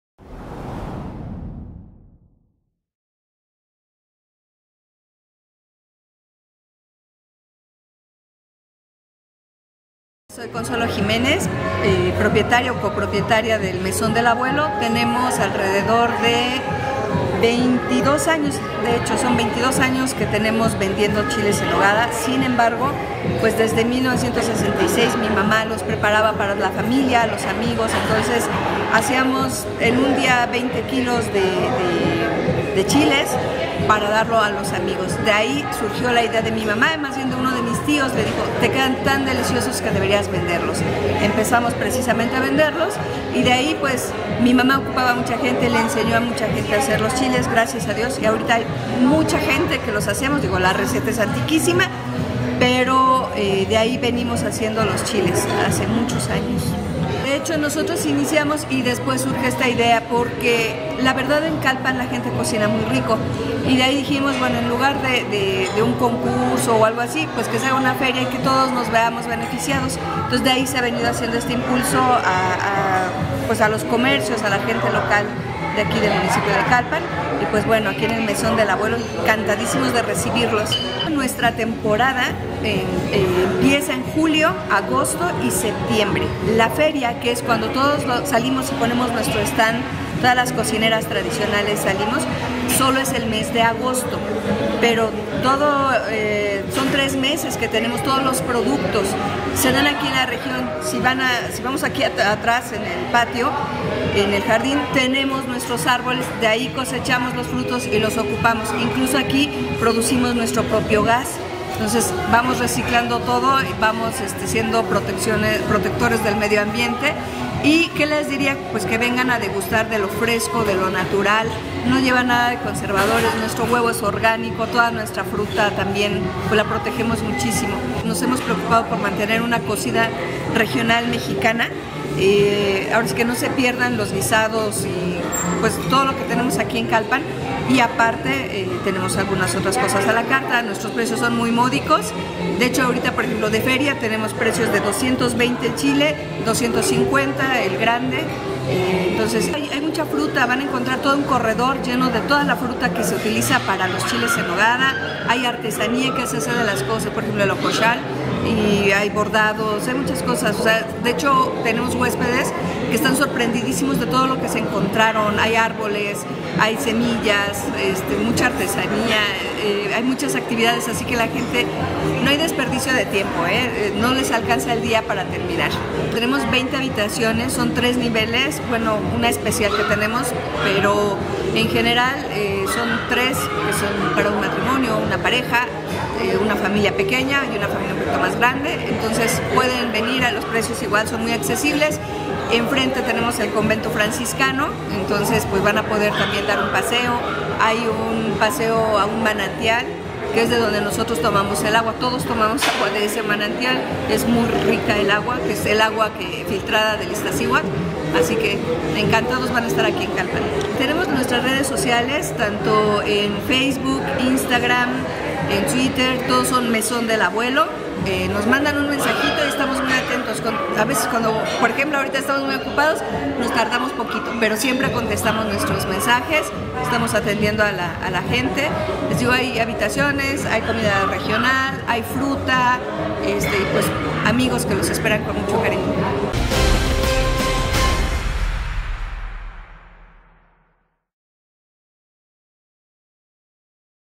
Chiles en Nogada en El Mesón del Abuelo en Calpan, PUEBLA.
Turismo | Si visitas Calpan, no dudes en alojarte en Mesón del Abuelo donde además podrás disfrutar de un delicioso ChileEnNogada durante esta temporada; desde hace 22 años ofertan este rico platillo poblano.